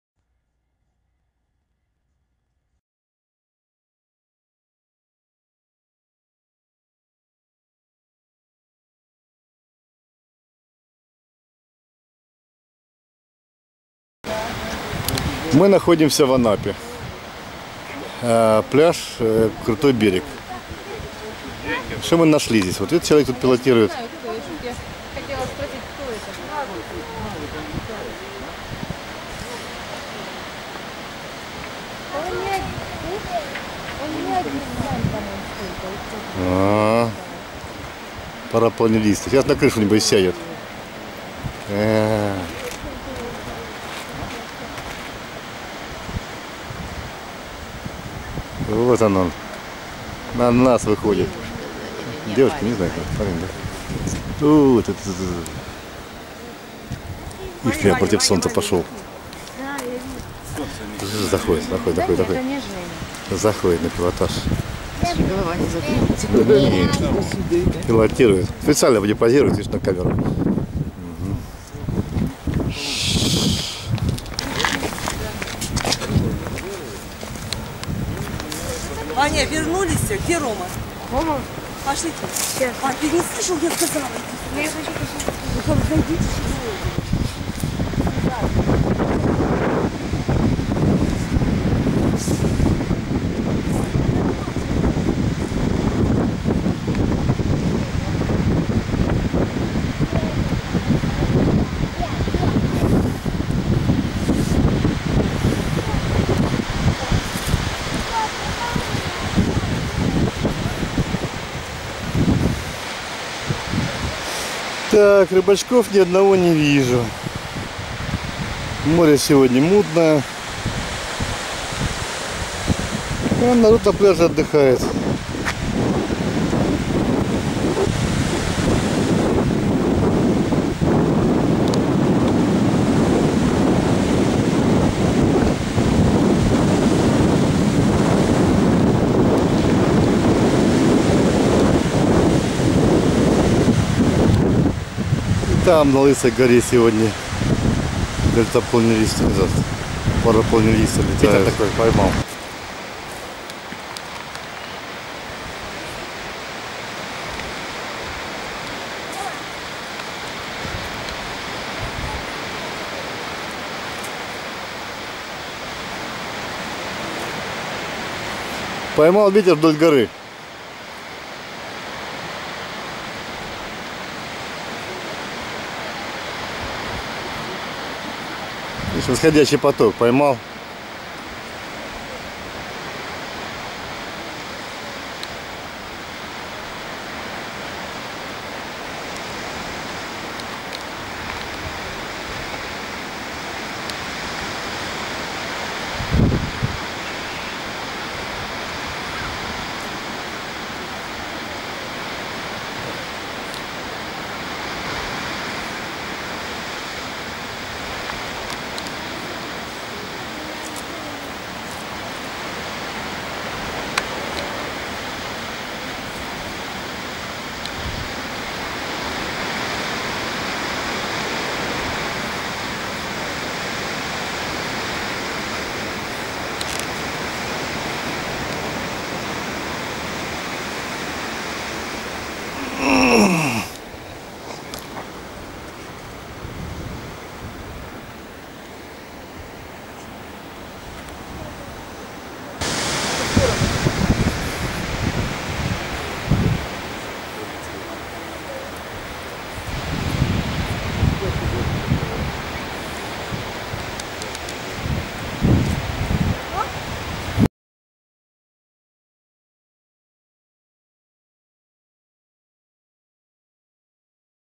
г. Анапа. Парапланерист над пляжем
г. Анапа. 4 ноября - двойной праздник. Народ отдыхает и летает  тоже... над пляжем...